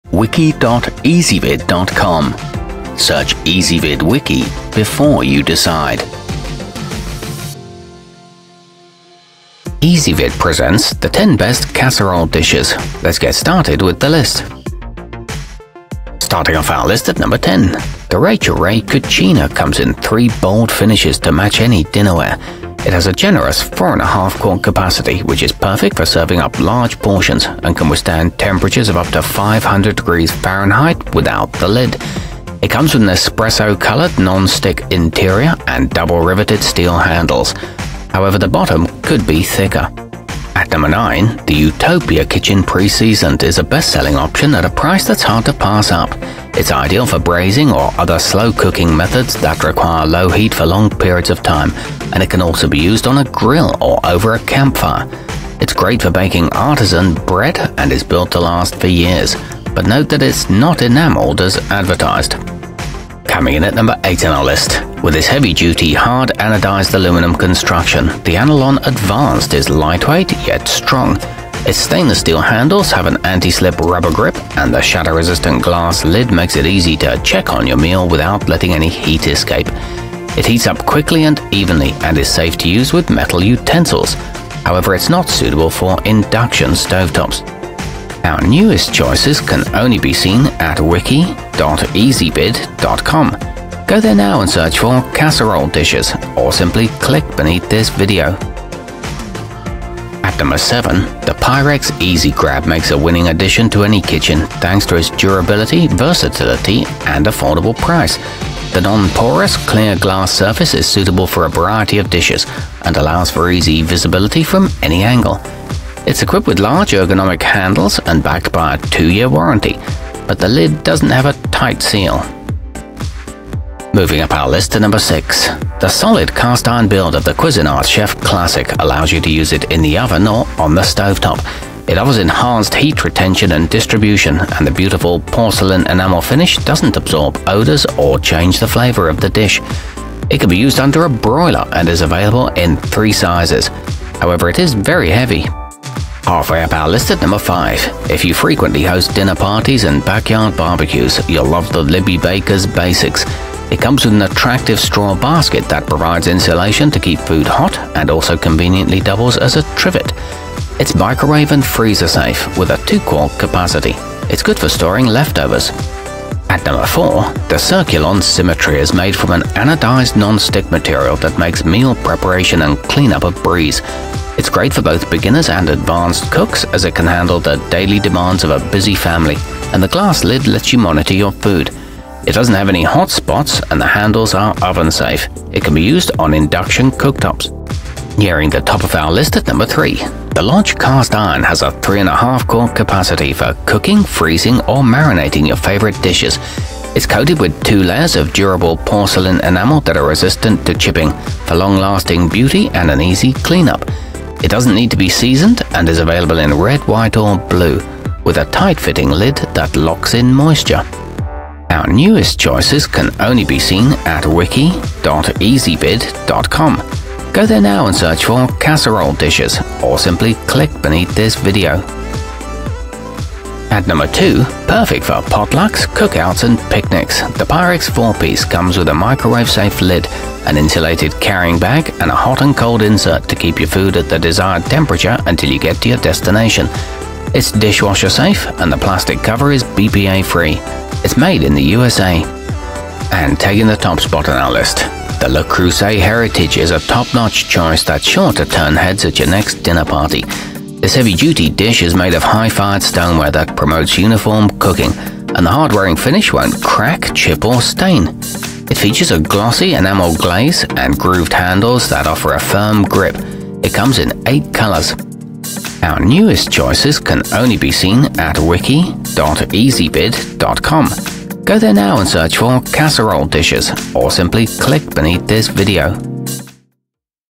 10 Best Casserole Dishes 2019
Our complete review, including our selection for the year's best casserole dish, is exclusively available on Ezvid Wiki.

Casserole dishes included in this wiki include the rachael ray cucina, utopia kitchen pre-seasoned, pyrex 4-piece, libbey baker's basics, pyrex easy grab, lodge cast iron, cuisinart chef classic, circulon symmetry, anolon advanced, and le creuset heritage.